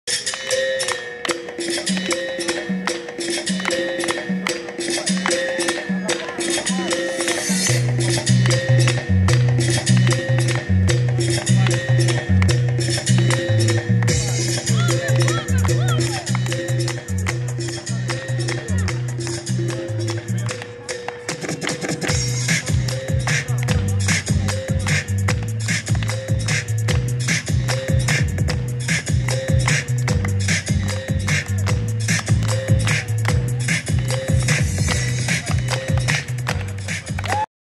«Музична акварель» у Вінниці 28.06.2014.
В рамках «Мистецьких вихідних»28 червня о 18 годині на майдані Незалежності відбулась святкова«Музична акварель». До Дня Конституції України вихованці «ДивоСвіту» розважали глядачів співом і танцями.